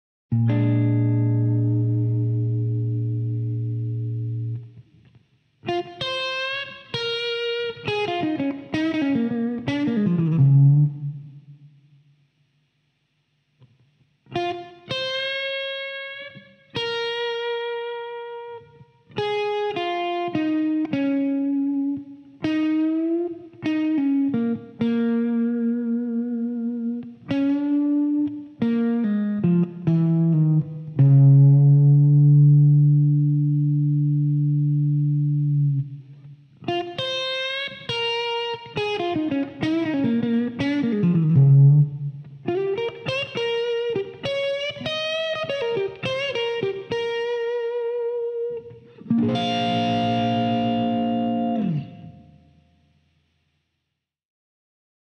Kirk Fletcher - Blues lick in B♭ | Guitar Lesson | #590
Very Short & Easy Blues Guitar Solo Lesson incl TAB & SLOW.